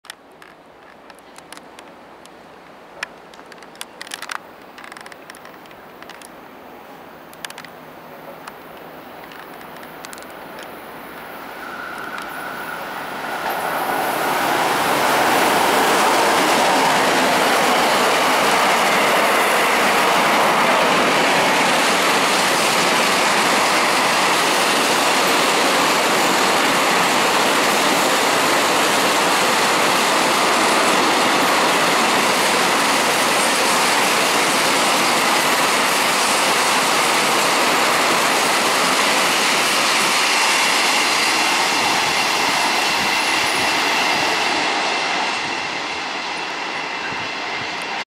스크린도어 설치 전의 오산대역을 통과하는 무궁화호와 화물열차
스크린도어 설치 전의 오산대역을 통과하는 무궁화호와 화물열차 (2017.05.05)